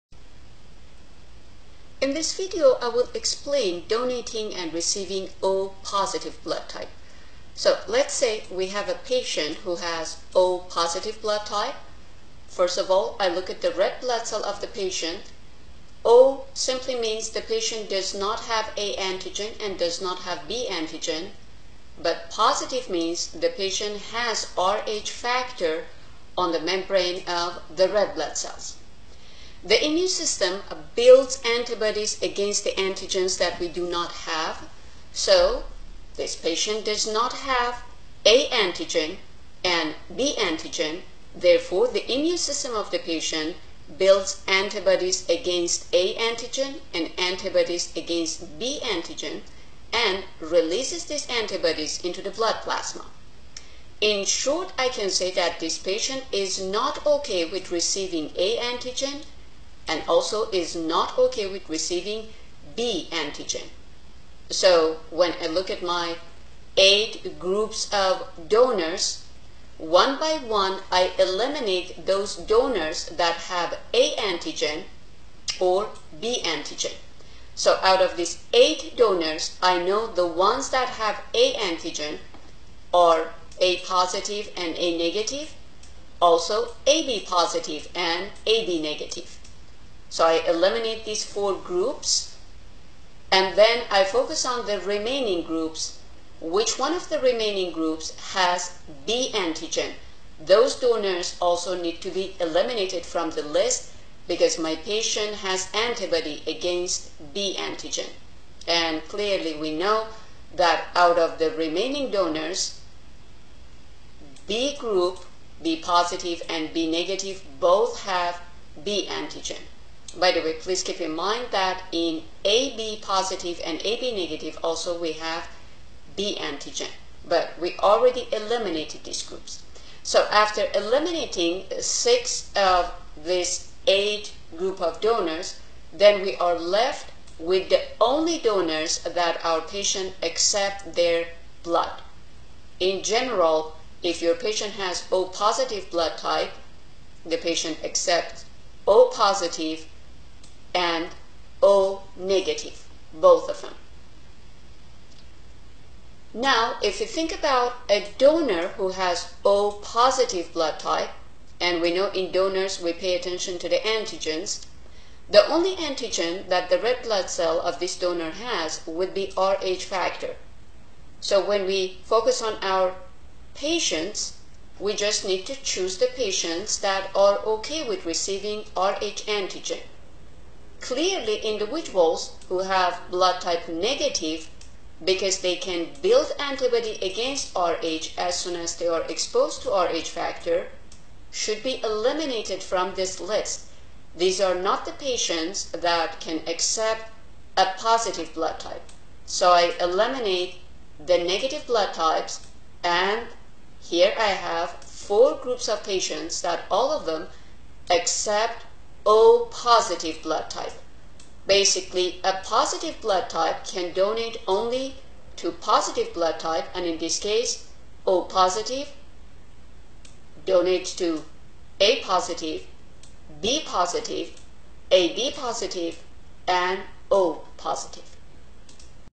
O Positive Blood Type (Donating and Receiving)
In this video, I will explain donating and receiving O positive blood type. I will explain a patient with O positive accepts which major blood types, and a donor with O positive blood type can donate to patients with which major blood types.